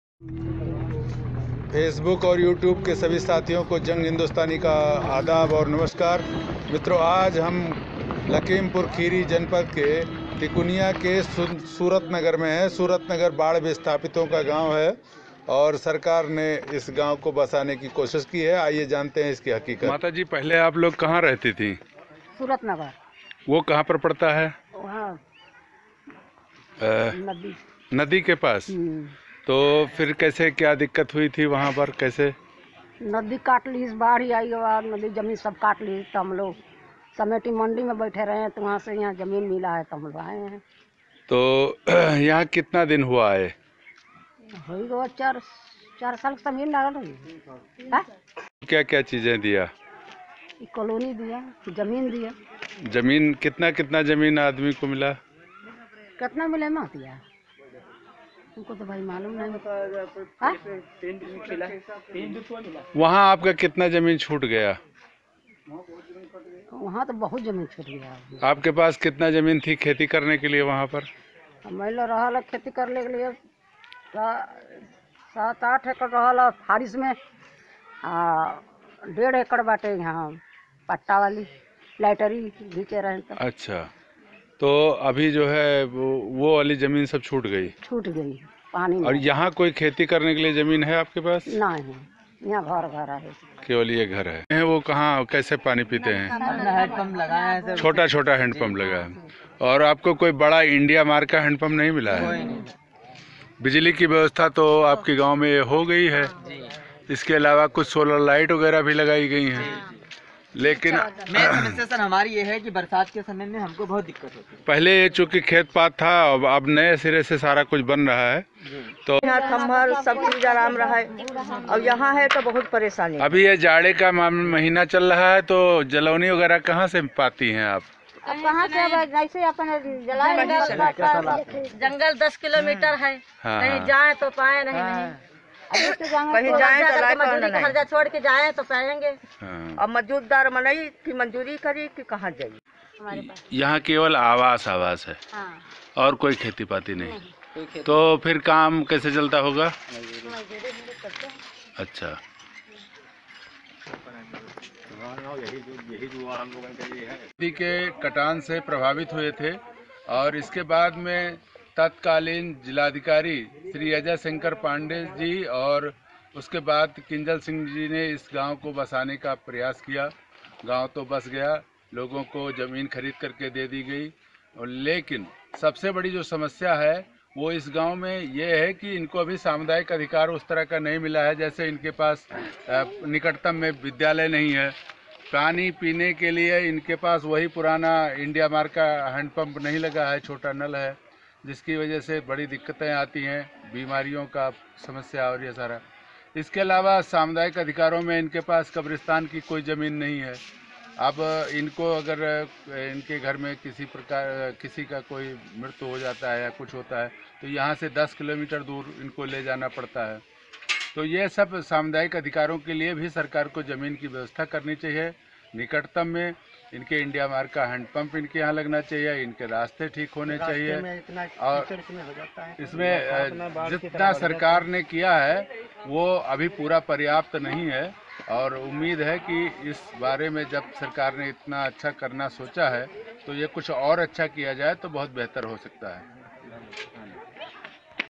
यूपी सरकार ने किया बेहतर काम - तिकुनिया, खीरी
लखीमपुर खीरी के तिकुनिया में कौड़ियाला नदी के करीब में बसे सूरत नगर जोकि बाढ़ में विस्थापित हो गए थे और तिकुनिया में मंडी की जमीन पर रह रहे थे।
 अखिलेश सरकार ने और उनके समय के तत्कालीन जिलाधिकारी किंजल सिंह की पहल पर जमीन खरीदकर 402 परिवारों को दिया गया।
 अभी तक केवल आवास की जमीन मिल सकी है अभी पेयजल विद्यालय तथा संपर्क में मार्गों का निर्माण होना बाकी है।
कुल मिलाकर सरकार ने एक अच्छा कार्य करने की कोशिश की।